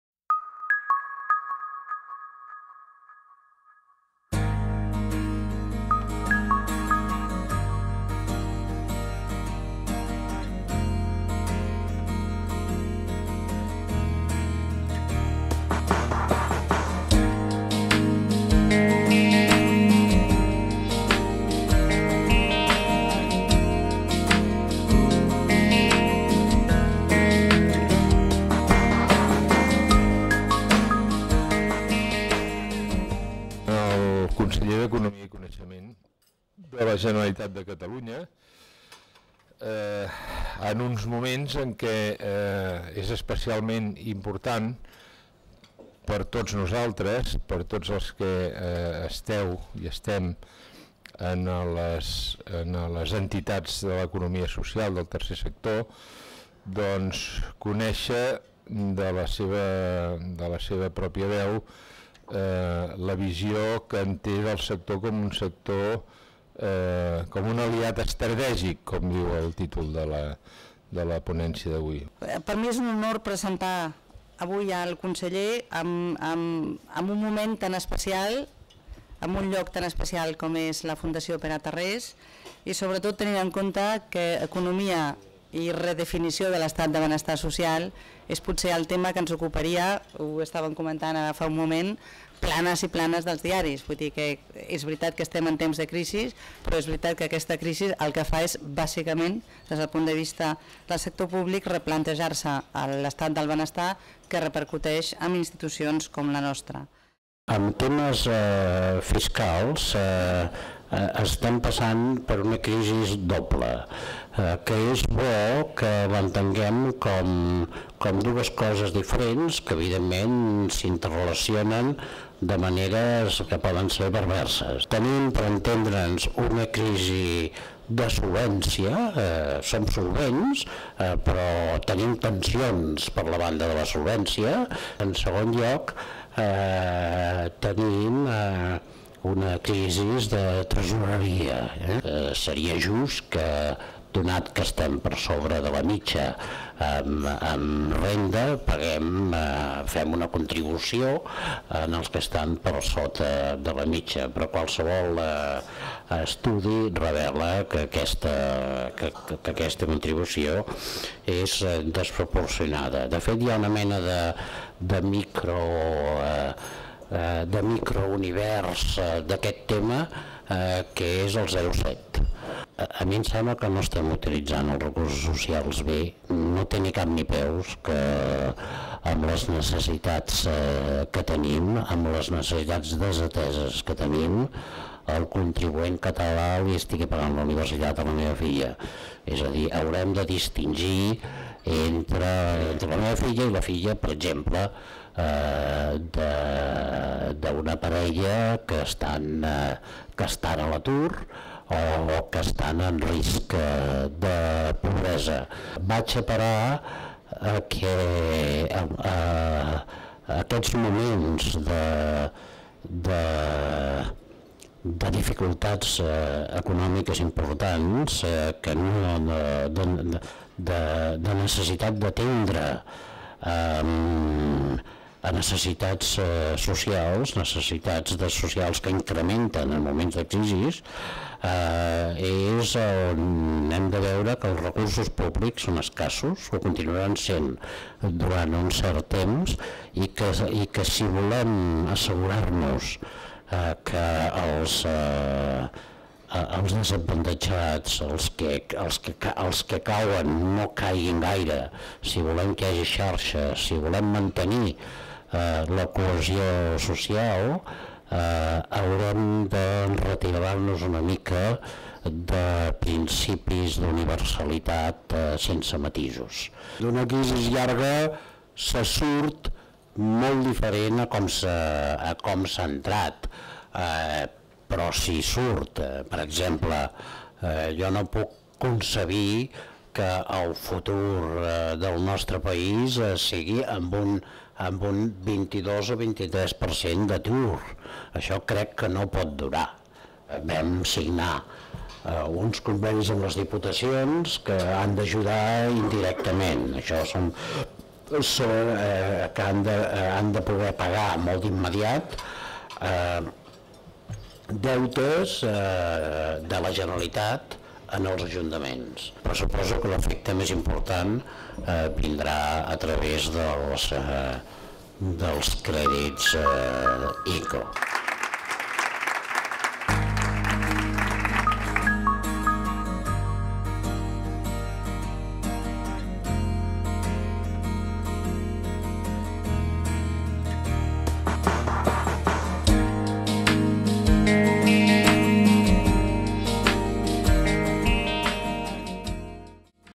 Fundació Pere Tarrés: Fòrum Social amb Andreu Mas-Colell
Andreu Mas-Colell, conseller d'Economia i Coneixement de la Generalitat de Catalunya, va intervenir en el Fòrum Social Pere Tarrés amb la conferència "El Tercer Sector Social: un aliat estratègic". En el debat posterior, el polític va respondre a les preguntes del centenar de persones, representants d'entitats socials i polítics, que van assistir-hi.